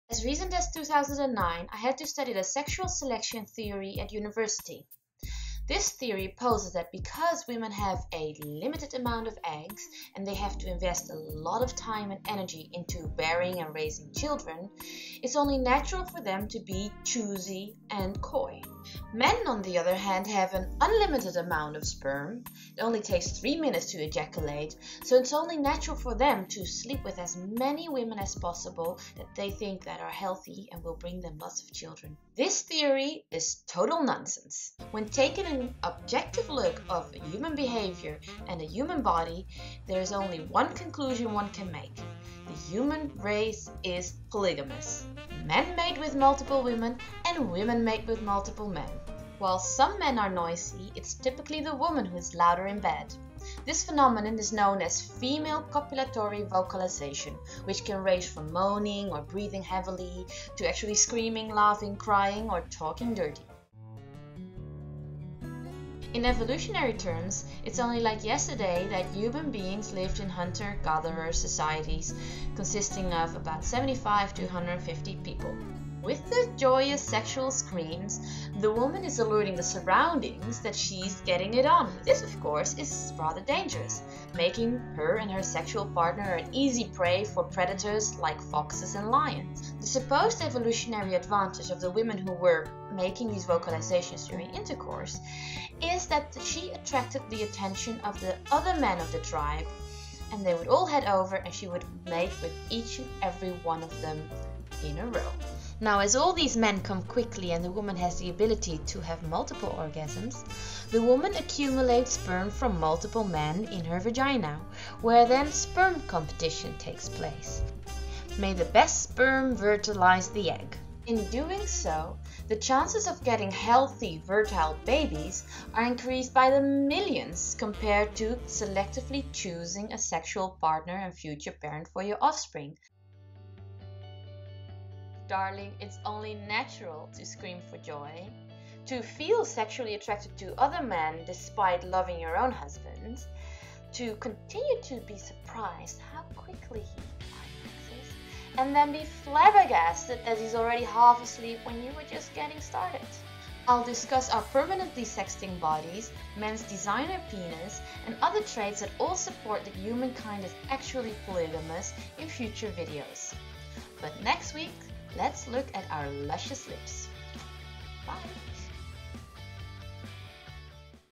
Why women are loud and men come early // Intimacy video #2
This video explains female copulatory vocalisation and sperm competition, which are two innate, natural human behaviours that indicate that human beings are polygamous.

Women breathing heavily, moaning, screaming or “talking dirty” during sexual activities is to attract more men to the scene. While men climax quickly, women can have multiple orgasms, promoting sperm competition.

This video defies the Sexual Selection theory and explains female copulatory vocalisation, polygamy, men’s tendency to ejaculate much faster than women, in a clear way.